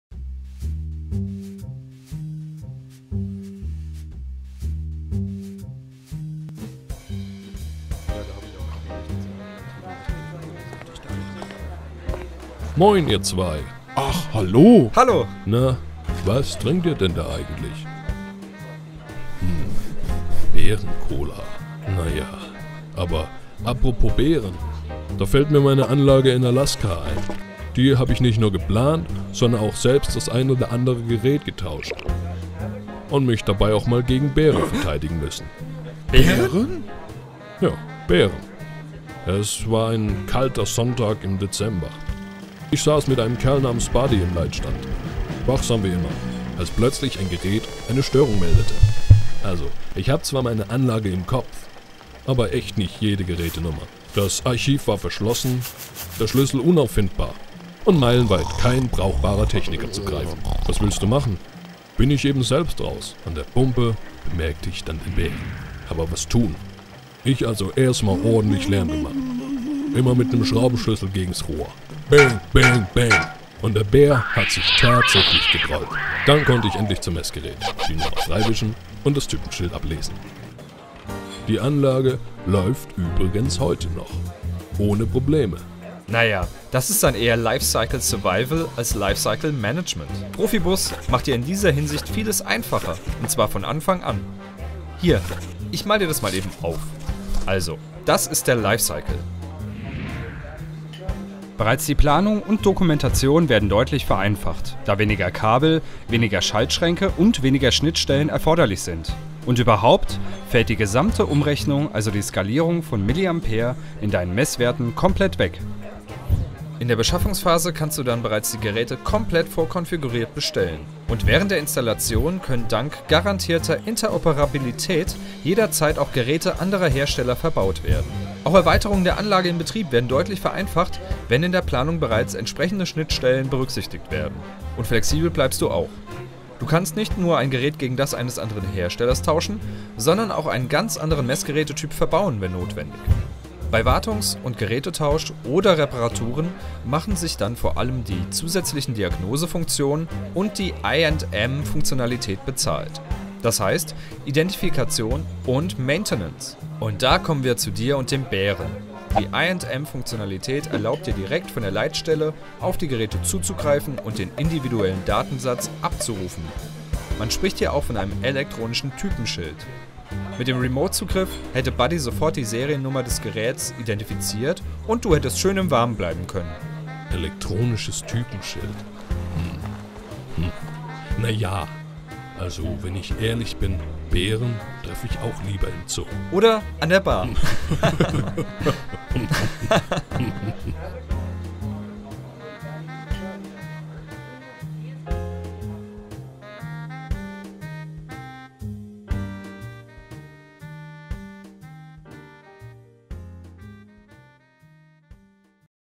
PROFIBUS Dialoge - Lifecycle Management / I&M-Funktionalität
Lifecycle Survival oder Lifecycle Management? In Folge 3 der PROFIBUS Dialoge geht es um Abenteuer, einen Bärtigen, einen Bären und die Vorteile von PROFIBUS im Lifecycle-Management einer Anlage.

Die „PROFIBUS Dialoge" setzen unsere Artikelserie zu PROFIBUS PA aus der Fachzeitschrift PROCESS fort. Der Clip gehört zur Ausgabe 6-2013